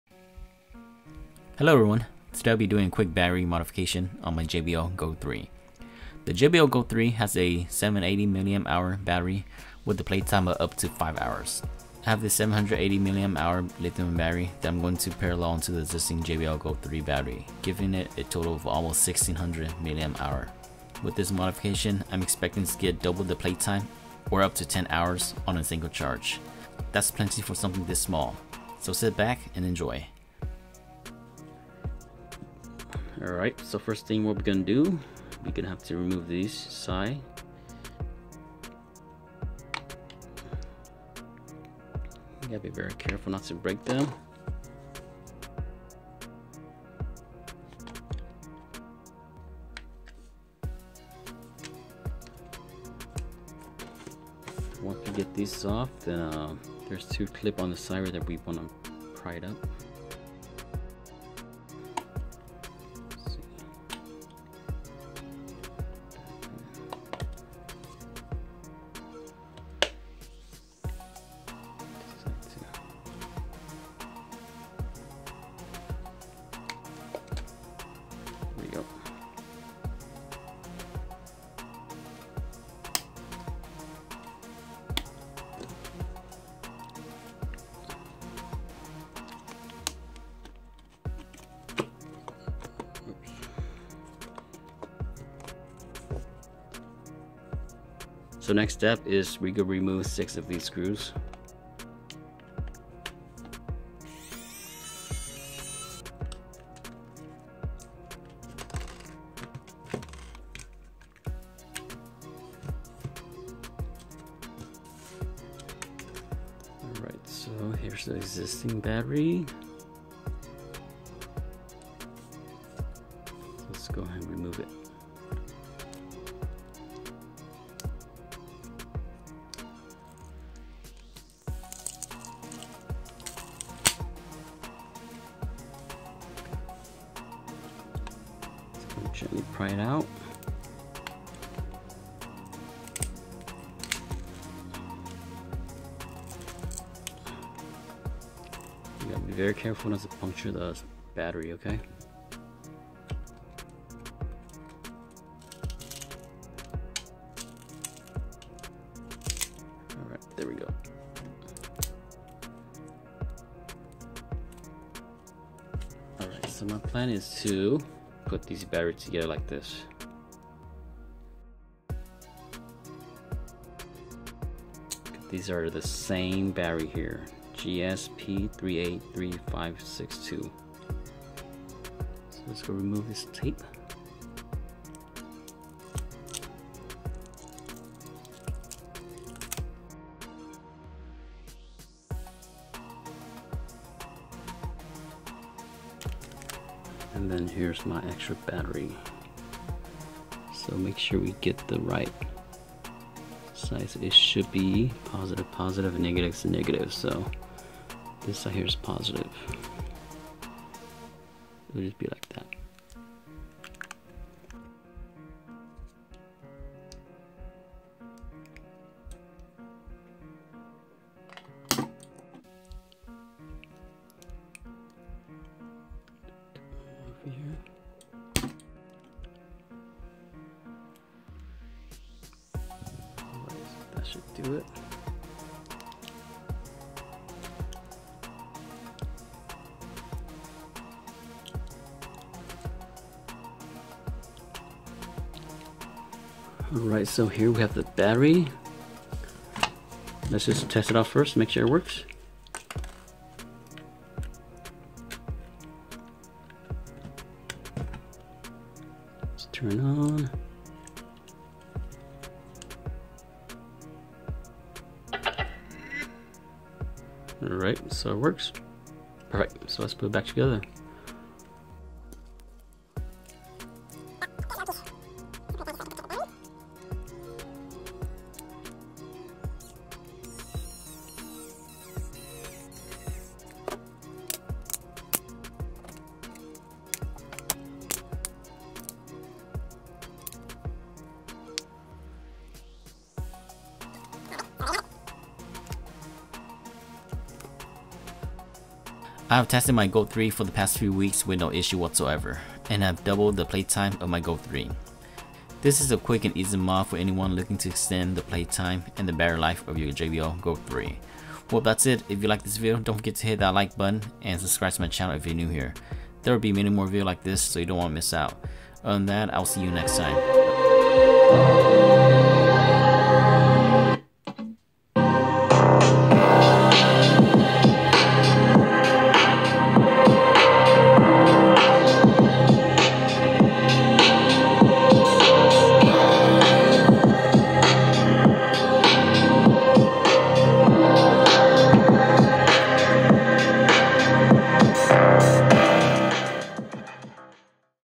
JBL GO 3 BATTERY UPGRADE [10+HRS PLAYTIME!!!]
Instead of the 5 hrs playtime on the JBL GO 3, why not double it up to 10 hrs of playtime.  Check this video out as I modified the battery of my JBL GO3 to to achieve a longer playtime on a single charge.  Enjoy!

JBL GO3
Electric screw driver
JBL GO Battery (383562)